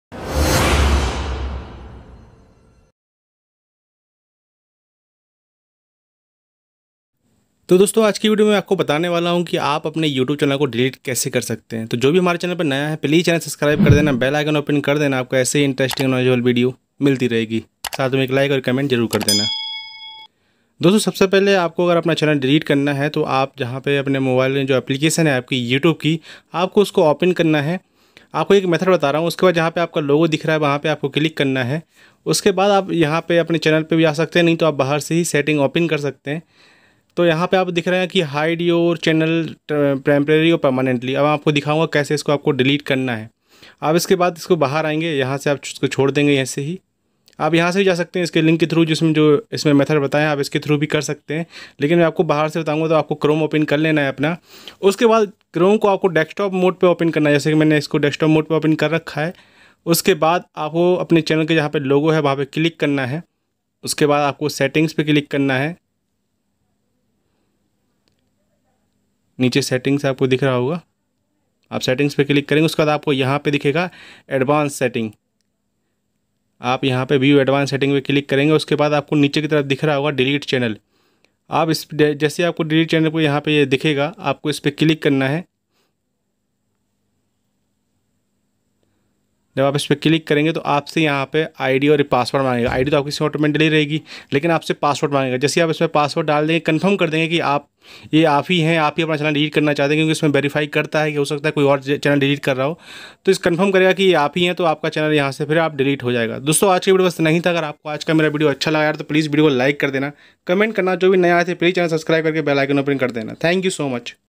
How to Delete YouTube Channel Permanently on Phone || The Viral Story
Title - How to delete YouTube channel permanently on phone
Guys if you want to delete your YouTube channel permanently then watch this video .
dosto agar aap search kar rahe hai ki YouTube channel ko kaise delete kare to aap sahi video per aaye hai .